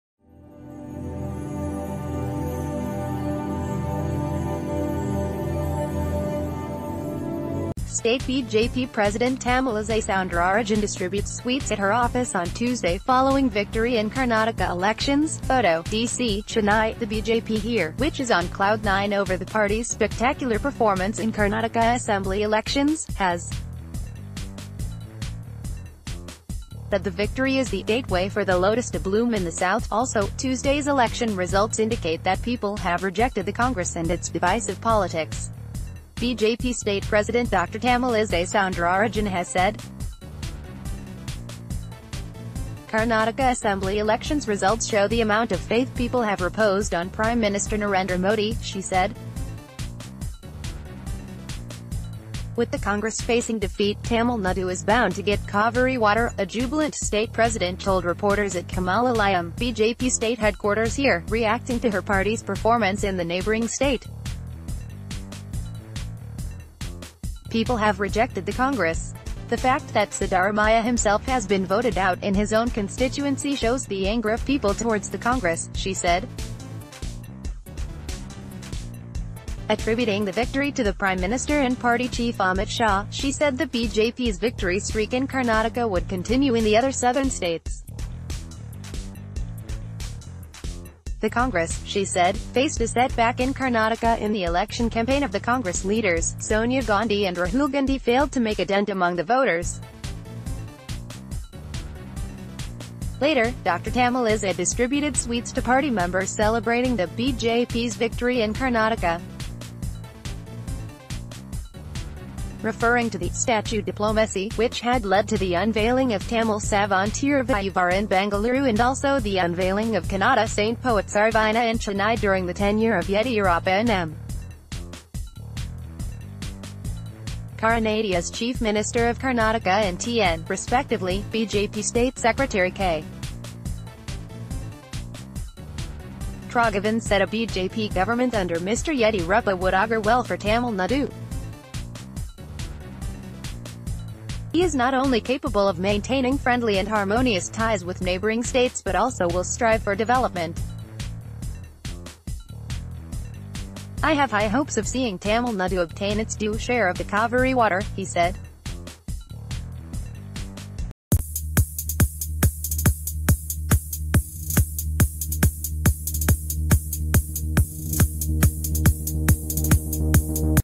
Breaking News | Karnataka BJP’s victory is gateway to south: Tamilisai Soundararajan
State BJP president Tamilisai Soundararajan distributes sweets at her office on Tuesday following victory in Karnataka elections (Photo: DC) Chennai: The BJP here, which is on cloud nine over the party's spectacular performance in Karnataka Assembly elections, has said the victory is the “gateway for the lotus to bloom in the South”.  Also, Tuesday's election results indicate that people have rejected the Congress and its “divisive politics,” BJP state president Dr Tamilisai Soundararajan has sa...